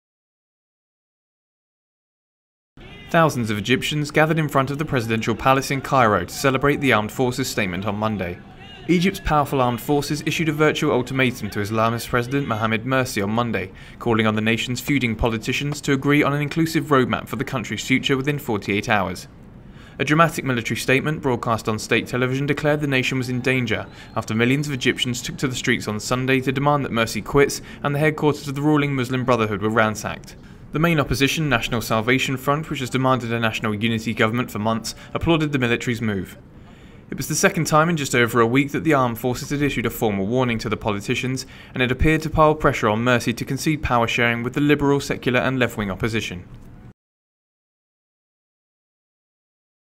Anti-Mursi Protesters Celebrate Army Ultimatum
Thousands of Egyptians gathered in front of the presidential palace in Cairo to celebrate the armed forces statement on Monday (July 1).